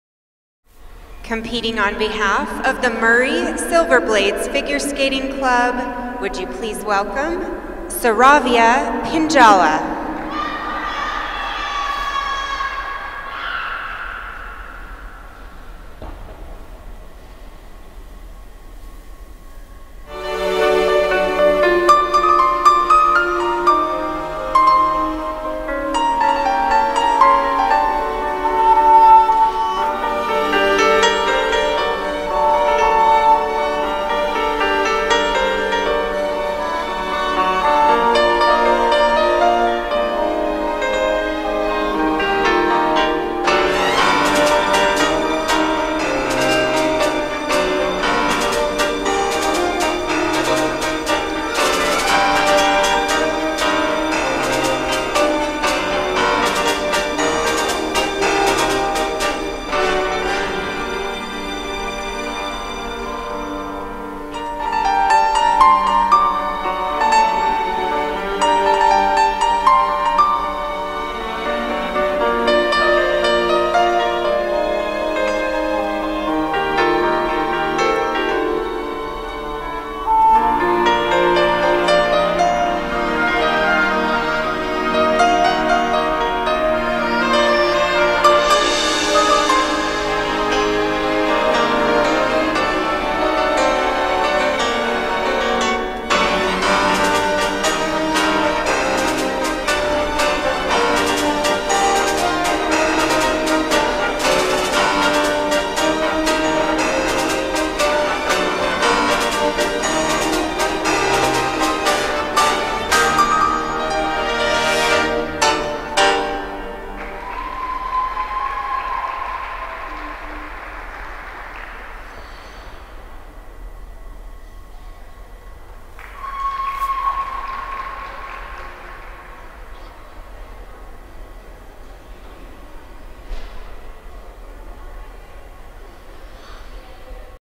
Sraavya Pinjala 2015 Intermountain Winter Classic ice skating
Sraavya won Silver medal.